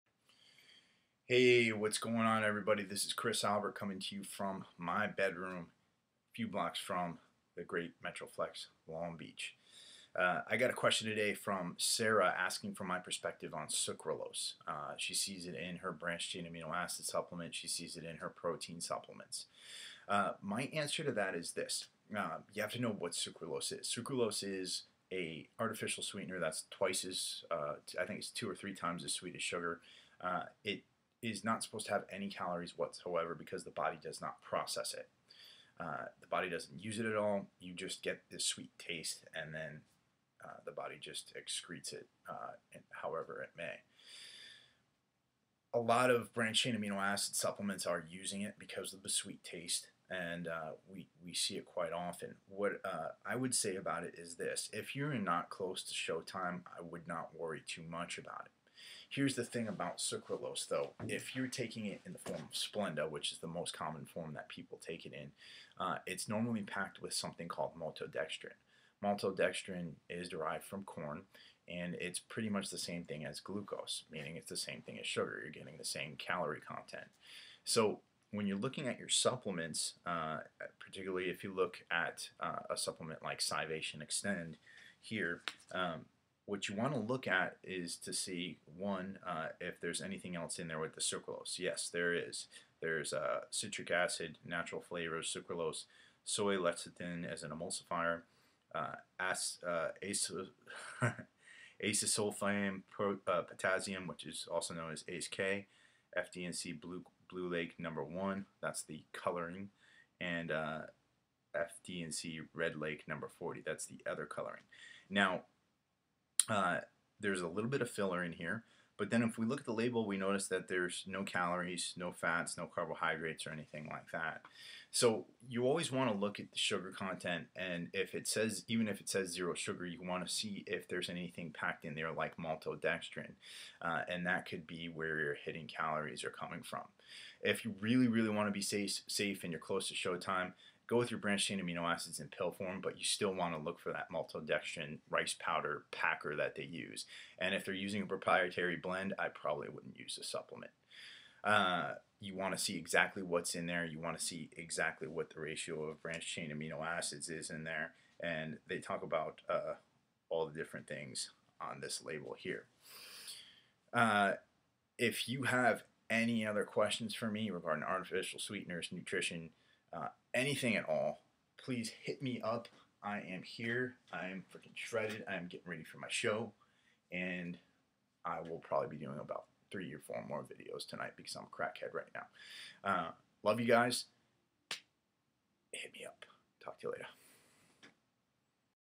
What's Hidden in Sucralose?
Not all supplements that have sucralose are bad, but some do carry a little surprise that you should know about.  With Chris Albert, Nutrition Specialist, Metroflex Gym Long Beach